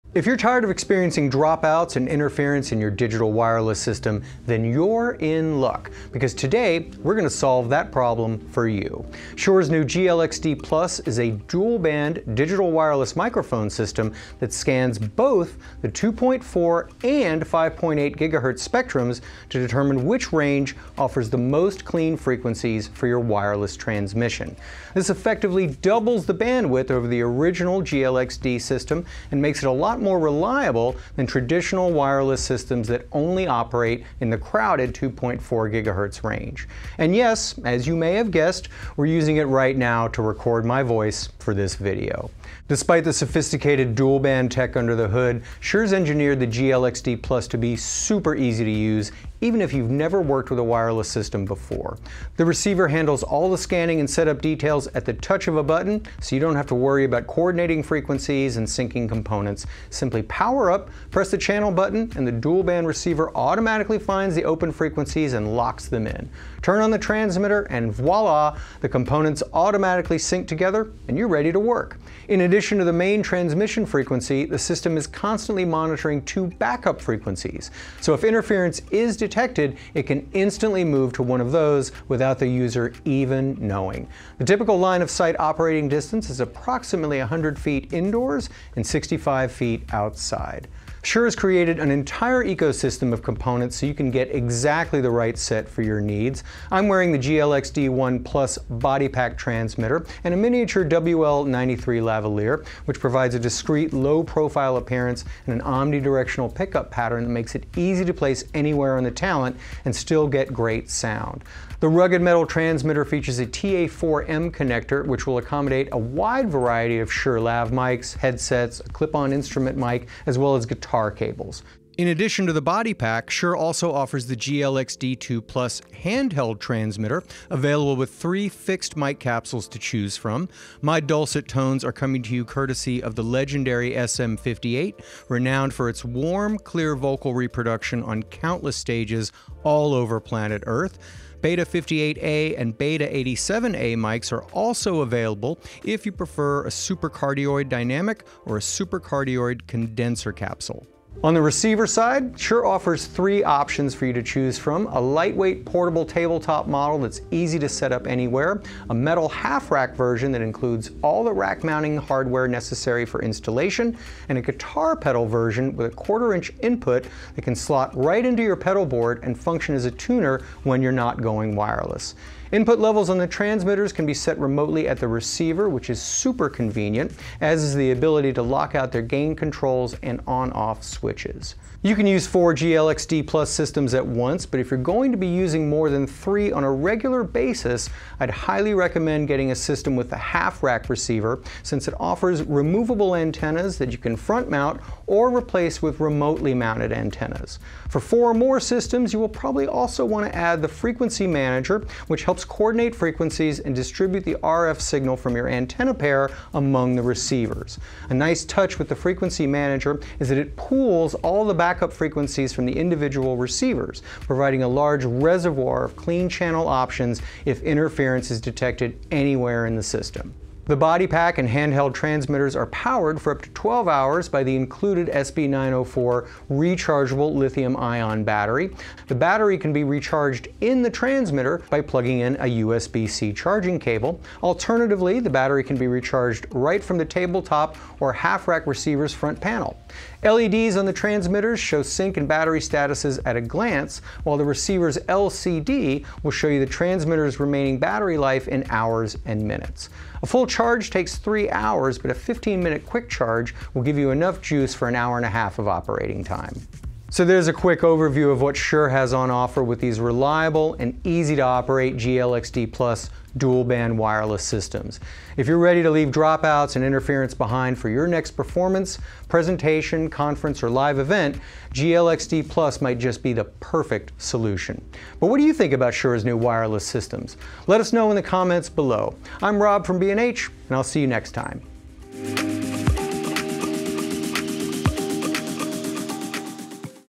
Shure GLX-D+ Dual-Band Wireless System
Tired of dropouts and interference in your digital wireless system? The Shure GLX-D+ is your solution with double the bandwidth of its predecessor, expanding into the 5.8 GHz range. The system also offers three different receiver types, an entire ecosystem of accessories, and versatile functionality for applications ranging from installed sound in small venues and live stage performances, to corporate facilities, educational institutions, and houses of worship.

Shop the Shure GLX-D+ Dual-Band Wireless System and Accessories at B&H

0:00 Specs
4:16 Final Thoughts